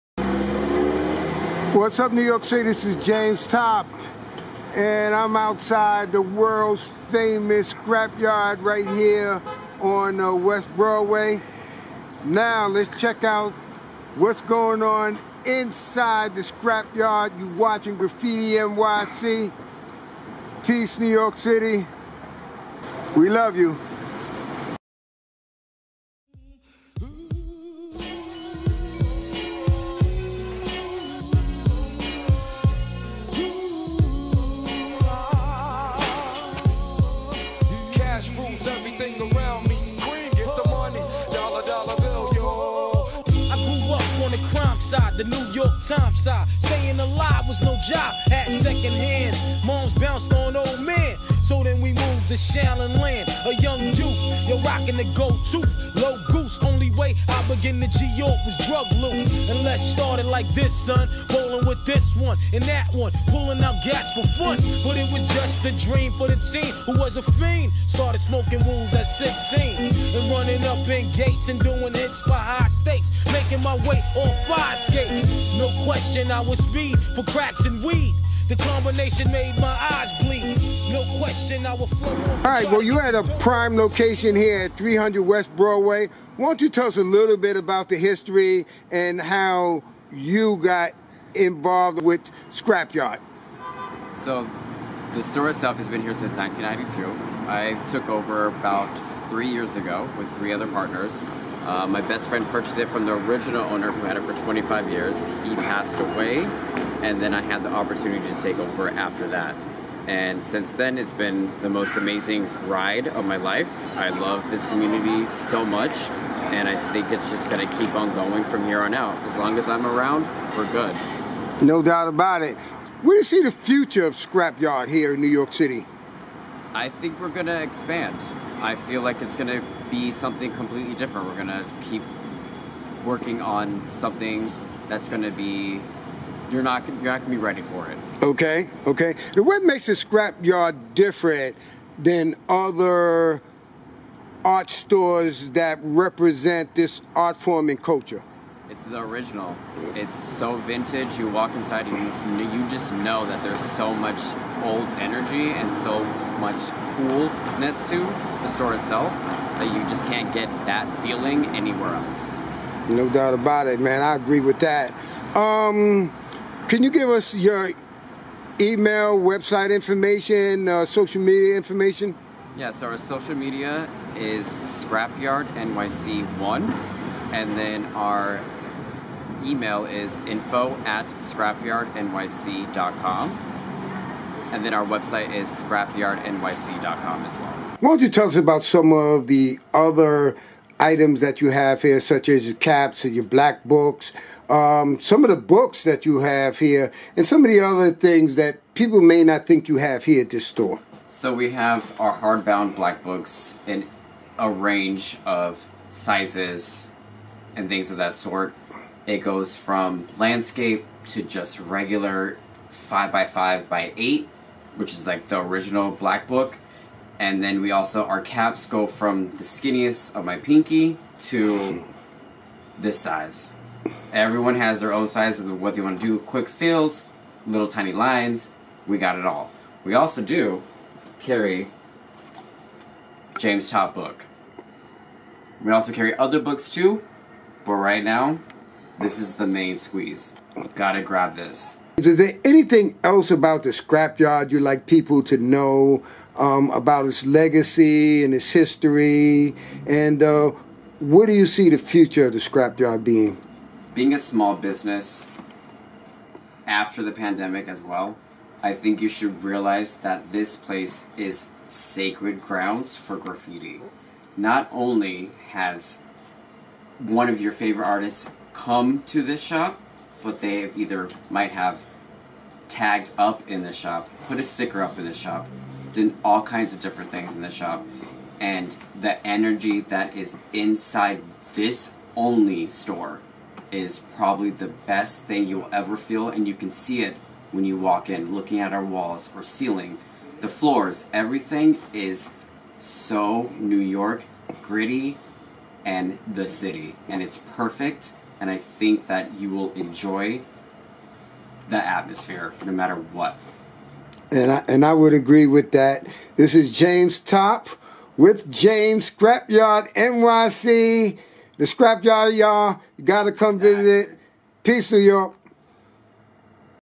Scrap yard nyc
Graffiti NYC TV Host James Top interviews owner of NYC graffiti art supplies store The scrap yard and what makes them so iconic. Located @ 300 west Broadway in NYC .
James E discuss his inventory and the future of the store and how to contact them on social media platforms.
EDIT BY ISIAH WALLLACE
CAMERA : JAMES TOP
PRODUCED BY: JAMES TOP PRODUCTIONS 2021  COPY RIGHTS